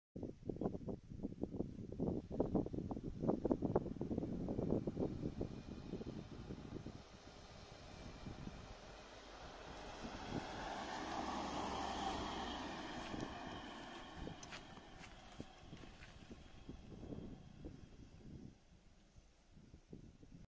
BMW E90, 318i in highlands of Sri Lanka
BMW e90 318i in approx 1500m above the sea level